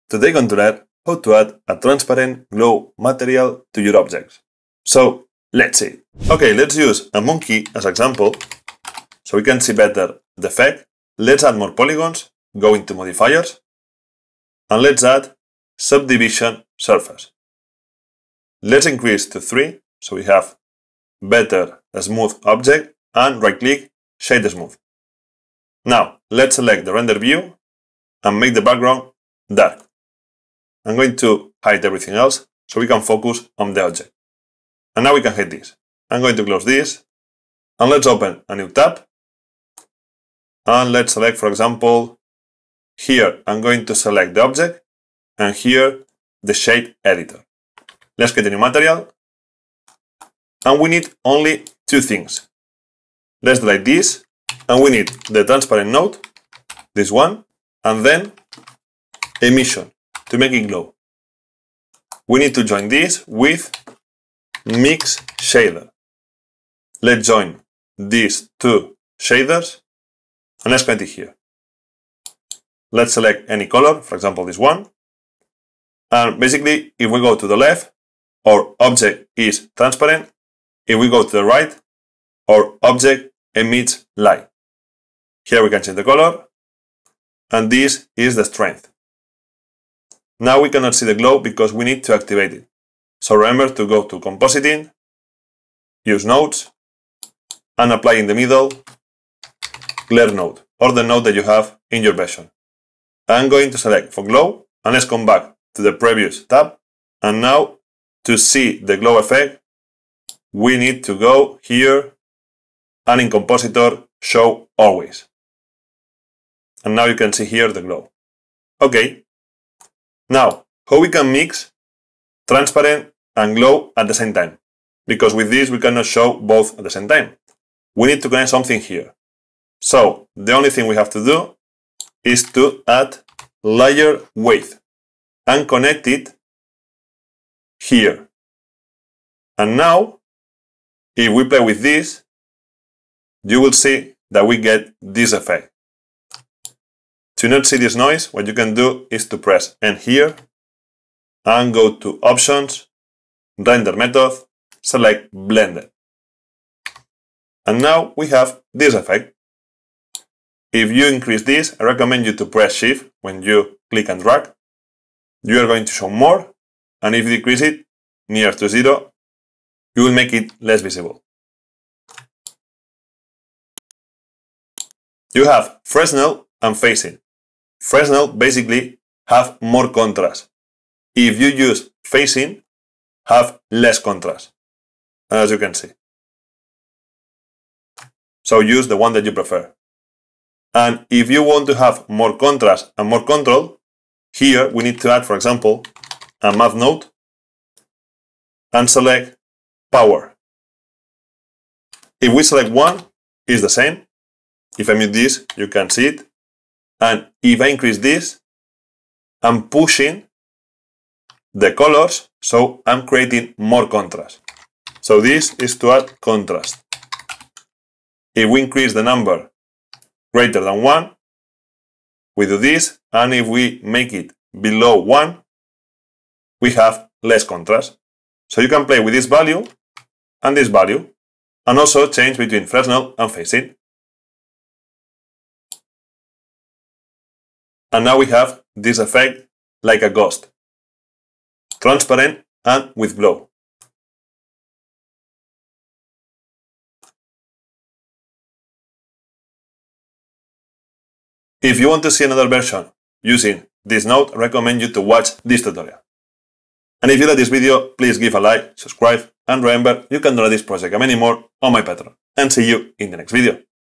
Easy Transparent Glow Material in Blender
Learn how to add a transparent material with glow effect to your objects in Blender. Like a hologram or ghost effect.

00:00 Intro
00:06 Use a smooth object
00:34 Transparent and Emission nodes
02:00 Layer Weight node
03:10 Add more contrast with Power